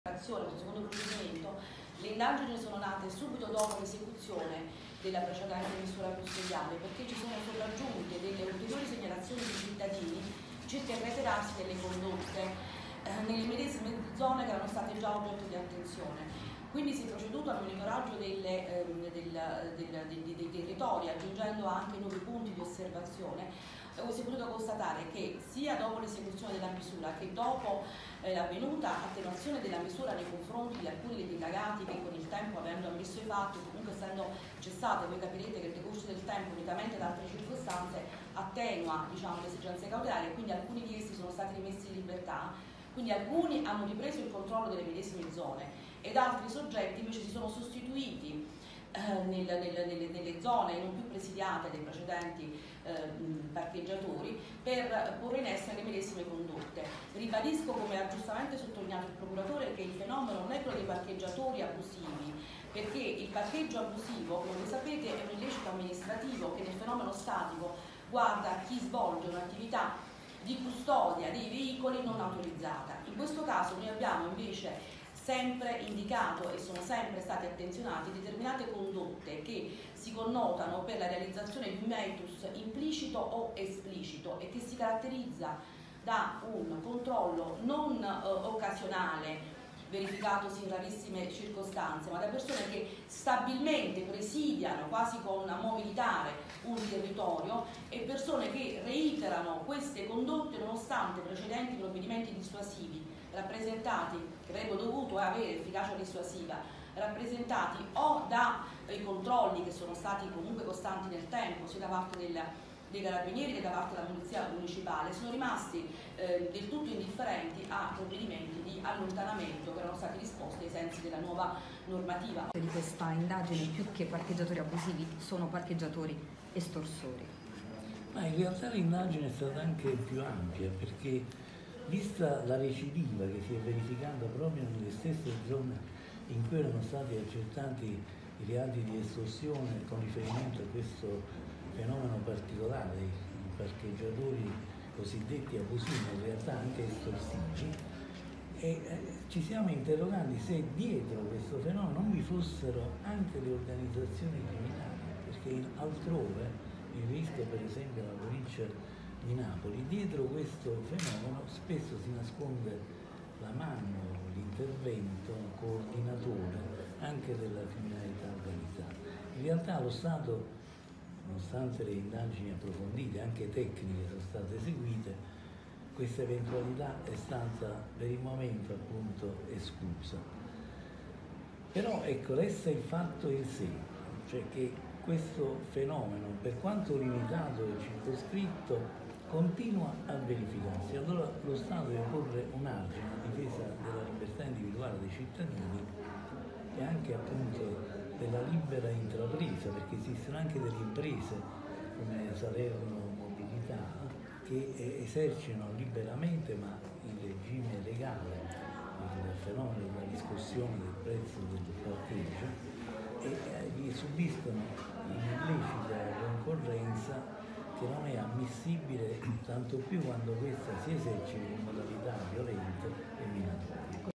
Salerno, la Procura arresta 32 parcheggiatori: "Sono estorsori"
Questa mattina i Carabinieri del comando provinciale di Salerno hanno eseguito un'ordinanza di custodia cautelare nei confronti di 32 indagati tra cui 6 in carcere, 24 ai domiciliari e 2 risultati irreperibili, responsabili a vario titolo di condotta estorsiva ai danni di automobilisti. Si tratta di parcheggiatori abusivi, alcuni dei quali già coinvolti in un precedente blitz. I provvedimenti restrittivi, presentati questa mattina in conferenza stampa dal procuratore della Repubblica Corrado Lembo, scaturiscono da un'indagine precedente del luglio 2017 su tutto il territorio cittadino. Nonostante i provvedimenti di allontanamento e quelli restrittivi già emessi, i reati sono stati reiterati fino all'ultimo blitz. Nel corso dell'indagine sono state accertate le condotte degli indagati finalizzate a costringere gli automobilisti a pagare somme di denaro anche con la violenza.